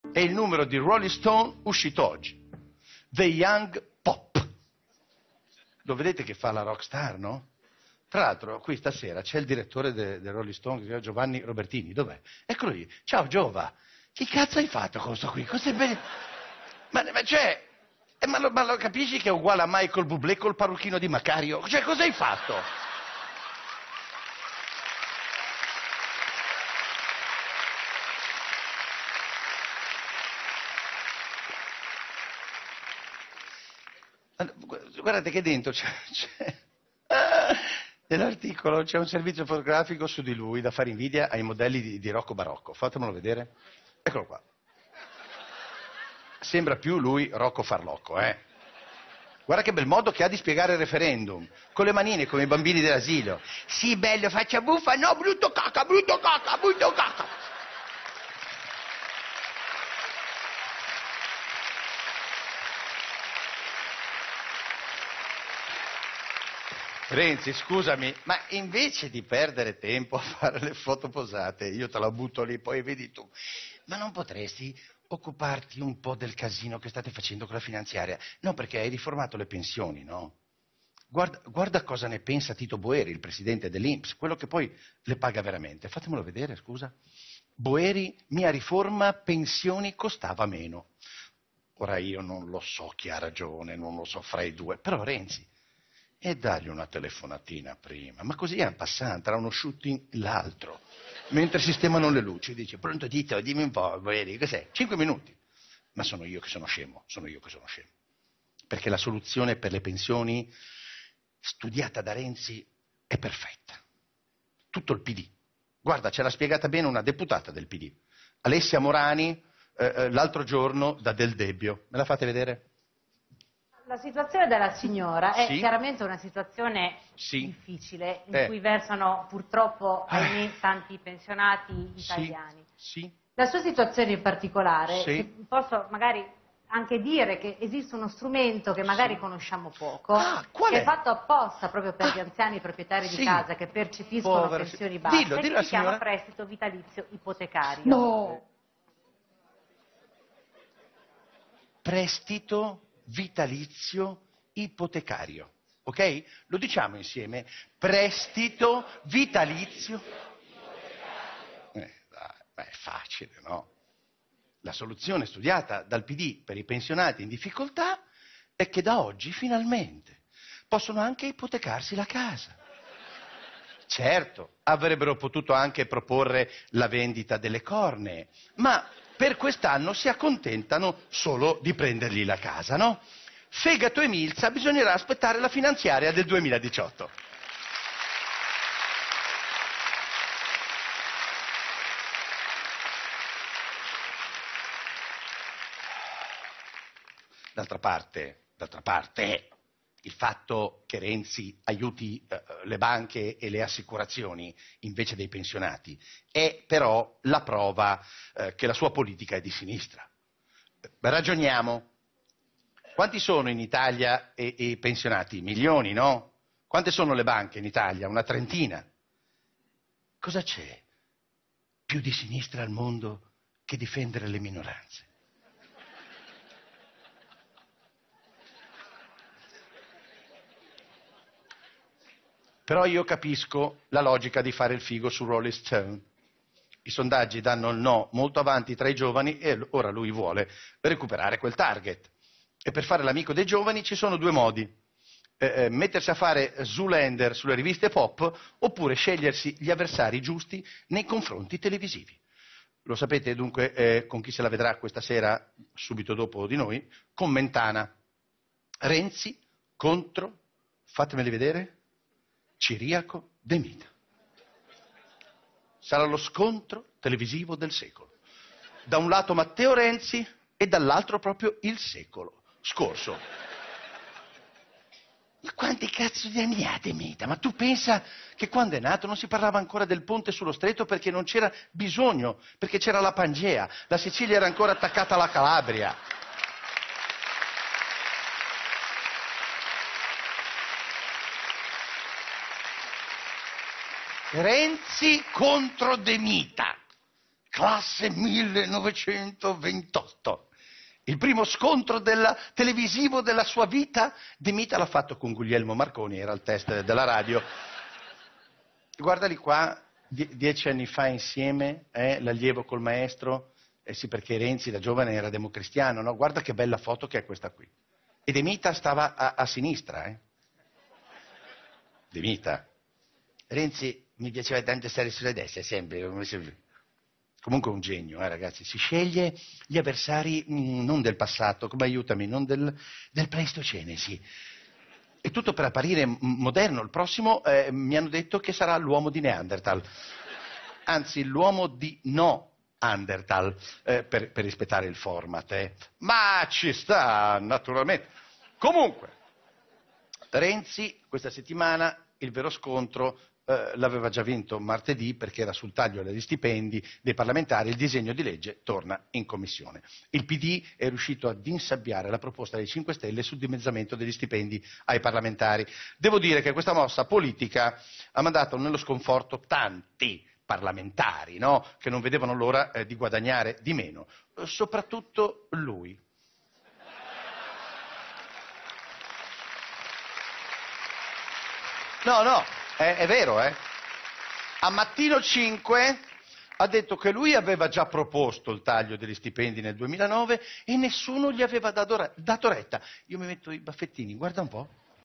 Il monologo politico di Maurizio Crozza
Il monologo dedicato alla politica di Maurizio Crozza dedicato Matteo Renzi intervistato da Rolling Stone